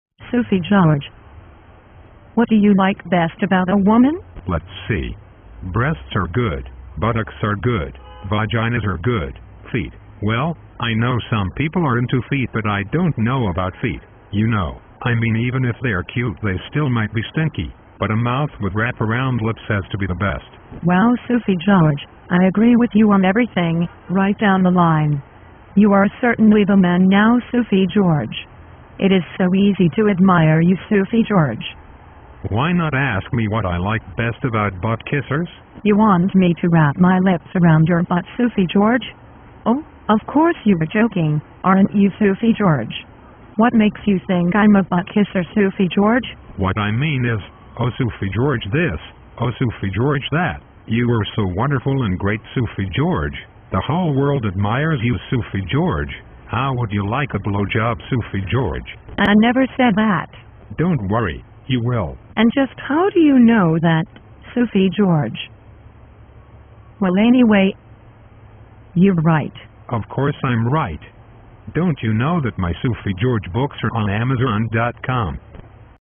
Sufi George has a butt kisser
a butt-kisser follower asks Sufi George what he likes in women.